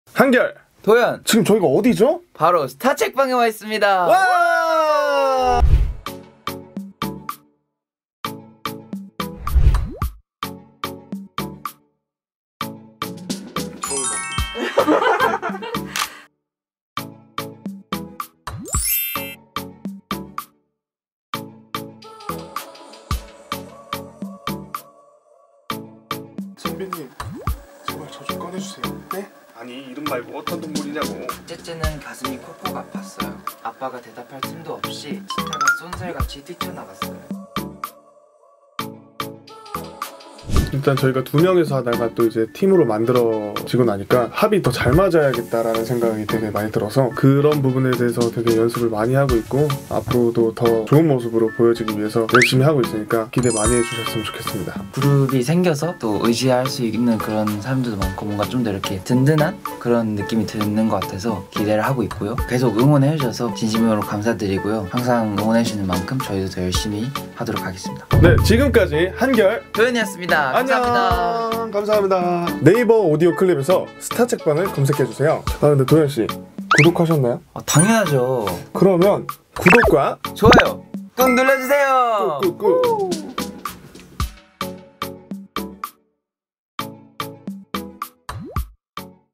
릴레이 재능기부 - BAE173 한결X도현 "기대 많이 해주실 거죠?" [스타책방]
[TV텐] 그룹 BAE173의 한결과 도현이 텐아시아와 네이버 오디오 클립이 주최한 '스타책방'에 스물다섯 번째 주자로 참여했다. '스타책방'은 스타들의 릴레이 재능기부 프로젝트로 청각장애 부모를 둔 아이들, 다문화 가정의 아이들에게 동화책을 읽어주는 취지로 제작됐다.

한결과 도현은 각각 그레이트북스 내 친구 과학공룡의 '날아라, 루루!'와 이야기 꽃할망의 '토끼의 재판'을, 뿡야와 친구들 시리즈의 '뿡야의 요술 씨앗'과 내 친구 과학공룡의 '푸카야, 사냥 가자'를 읽었다.

두 사람은 단숨에 동화 속 캐릭터에 몰입, 섬세한 표현력으로 완성도 높은 오디오북을 만들었다.

앞서 한결과 도현은 유닛 듀오 H&D로 활동을 마친 후 지난 19일 BAE173으로 돌아왔다.

이와 관련해 한결은 "두 명이서 하다가 팀으로 만들어지고 나니까 합이 더 잘 맞아야겠다는 생각이 들더라. 이런 부분에 대해 연습을 많이 하고 있다"면서 "앞으로 더 좋은 모습을 보여주기 위해 열심히 할 테니 기대 많이 해 달라"고 전했다.

도현은 "그룹이 생겨서 의지할 수 있는 사람들이 많아졌다. 그만큼 뭔가 든든한 느낌이 들어서 기대하고 있다. 계속 응원해주는 팬들에게 진심으로 감사하다"고 밝혔다.

'스타책방'은 매주 월, 목요일 네이버 오디오 클립을 통해 공개되며 네이버 부모i 판에서도 만날 수 있다.